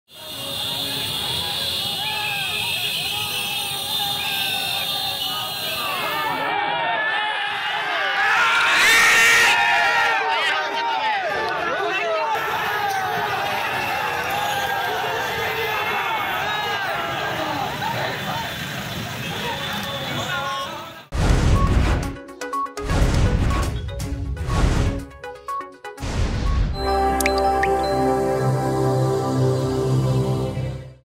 ମମିତା ହତ୍ୟାକାଣ୍ଡ ଘଟଣାକୁ ନେଇ ତେଜୁଛି ଅସନ୍ତୋଷ
ବିରୋଧୀ ବିଜେପି ପକ୍ଷରୁ ପ୍ରତିବାଦ କରାଯାଉଥିବା ବେଳେ ଆଜି ମୁହାଁମୁହିଁ ହୋଇଛନ୍ତି ବିଜେପି ଓ ବିଜେଡି କର୍ମୀ। ଘଟଣାସ୍ଥଳରେ ଦେଖାଦେଇଛି ଉତ୍ତେଜନା